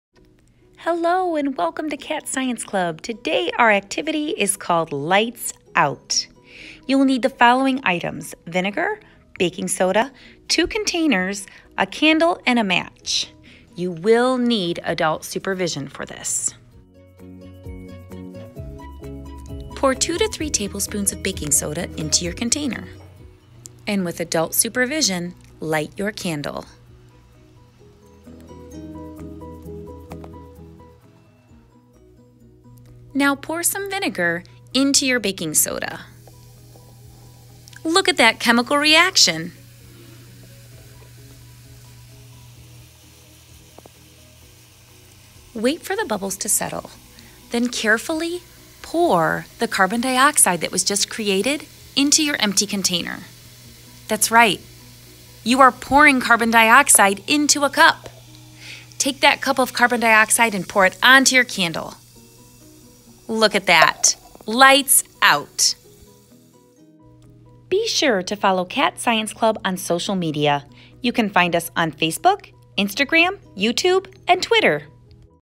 Lights Out / Mini Fire Extinguisher
Your own mini fire extinguisher. Using a chemical reaction to produce carbon dioxide we will put a candle's flame out. Or just pretend it's magic ;-)

Have an adult help.